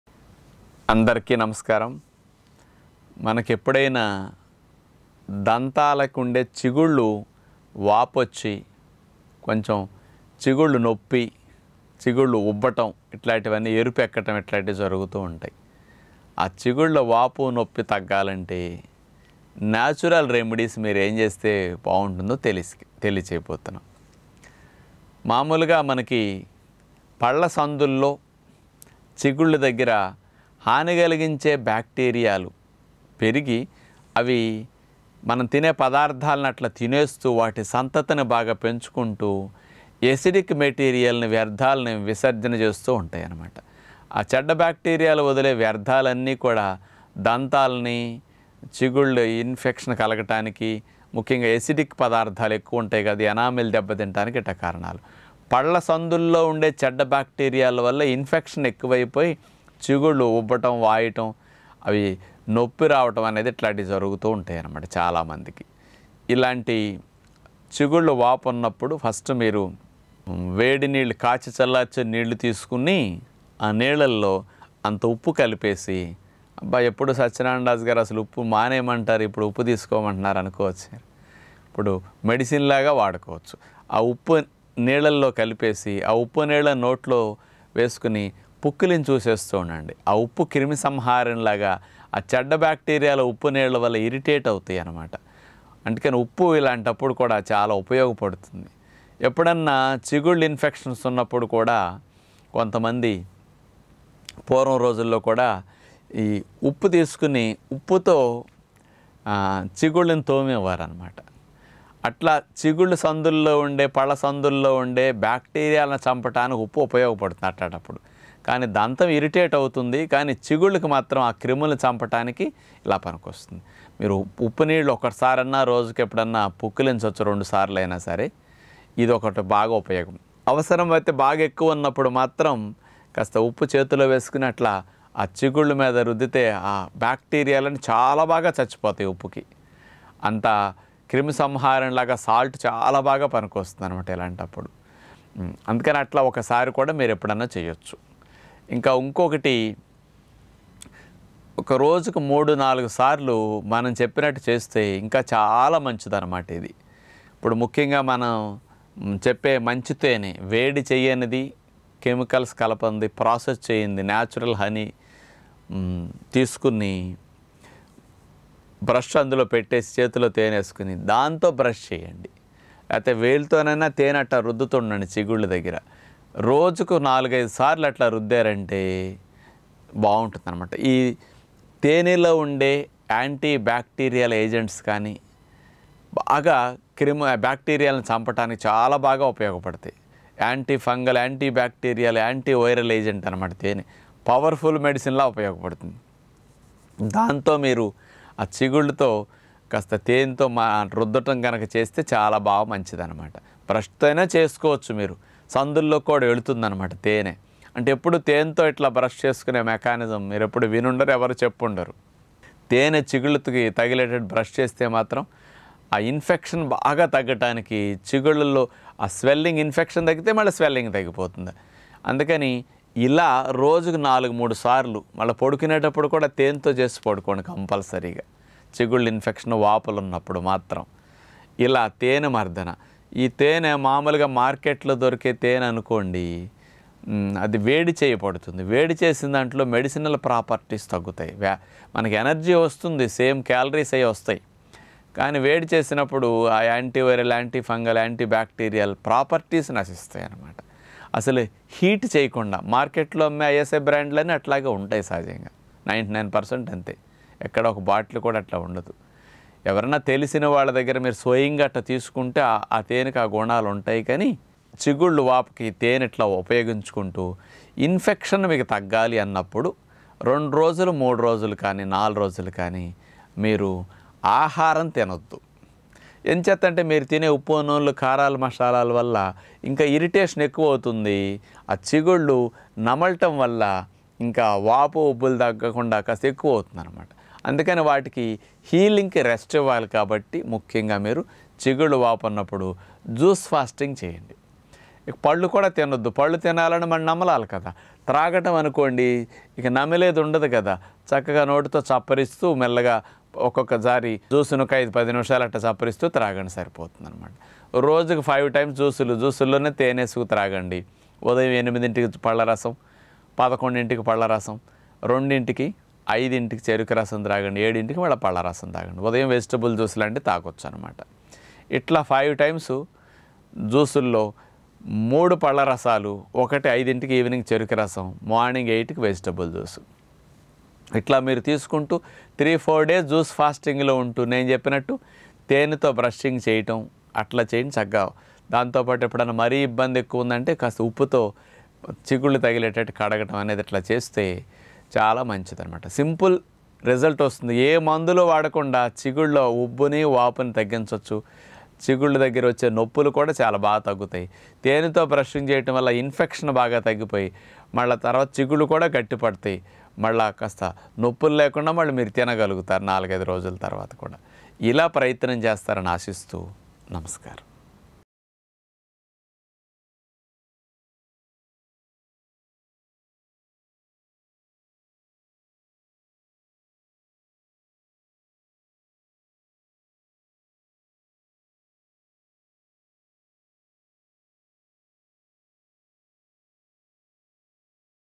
Home Remedy for Swollen Gums & Toothache | Protect Teeth Enamel | Manthena Satyanarayana Raju
5 Foods to Remove Weakness & Strengthen Your Body - నీరసాన్ని తగ్గించి బలాన్ని పెంచే అతి బలమైన 5 ఆహారాలు